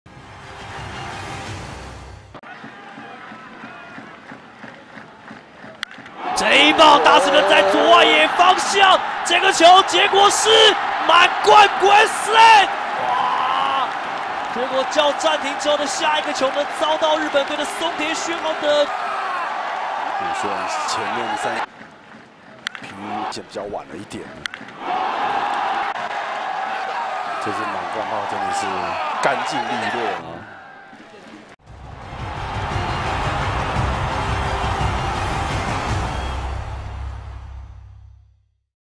Best Plays of the 2015 WBSC Premier12 - Nobuhiro Matsuda
🇯🇵 Nobuhiro Matsuda 松田 宣浩 (Fukuoka Softbank Hawks) hits the first GRAND SLAM in Premier12 history